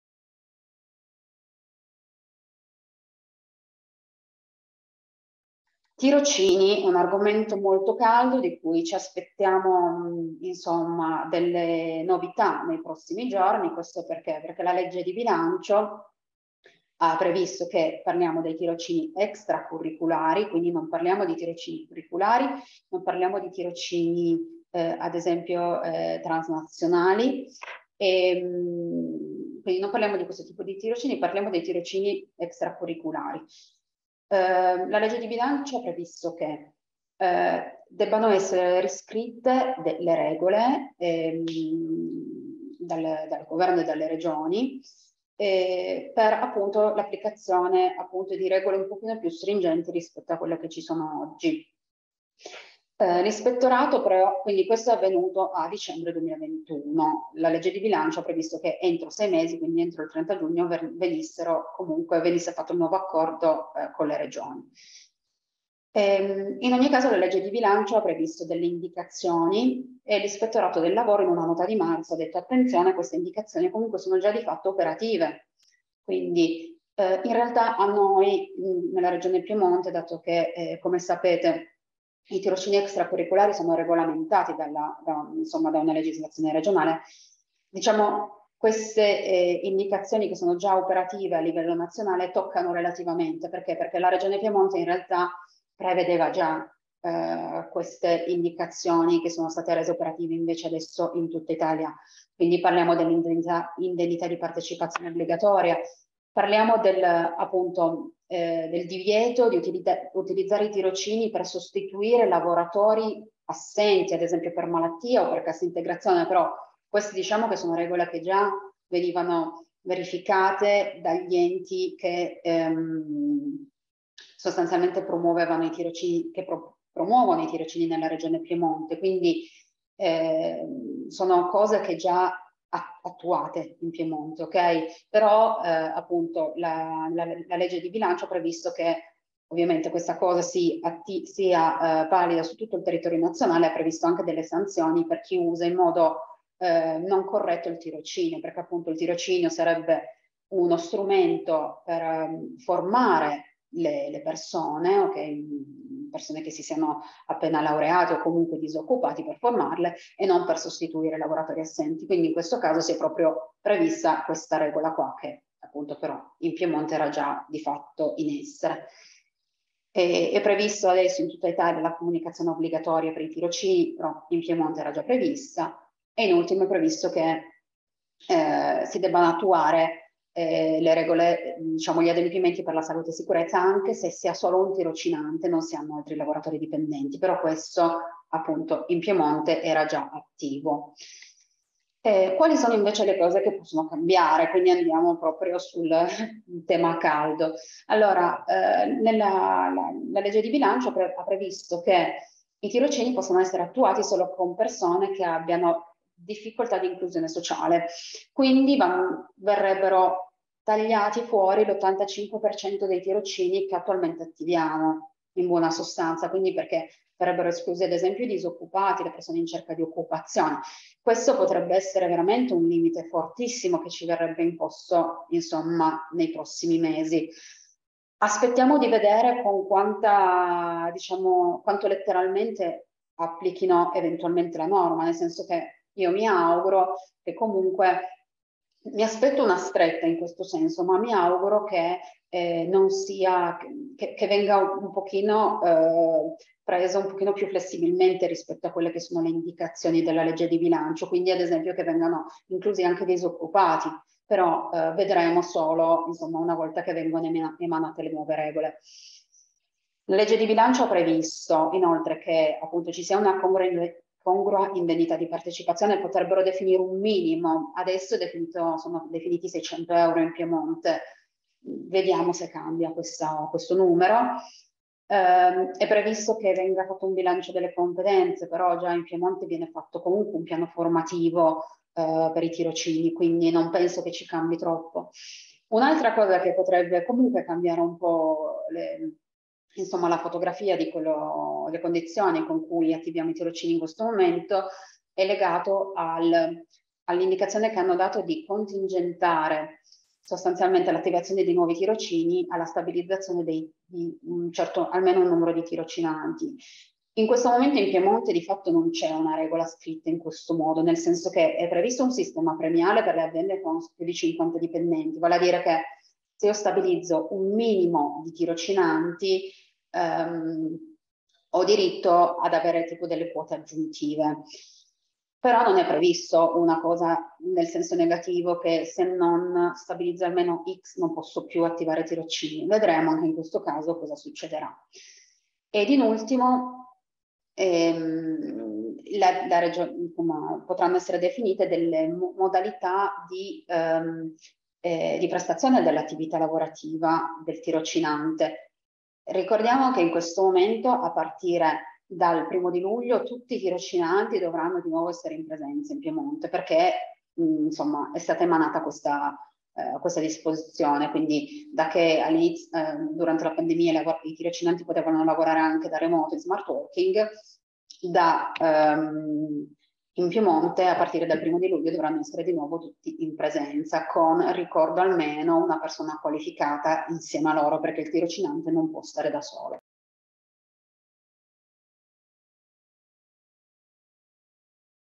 Tirocini: le prime novità in attesa dell'accordo Stato-Regioni - Pillole StudioMarino
Da Webinar 28 giugno 2022 - Laura Quassolo
In attesa dell'accordo Stato-Regioni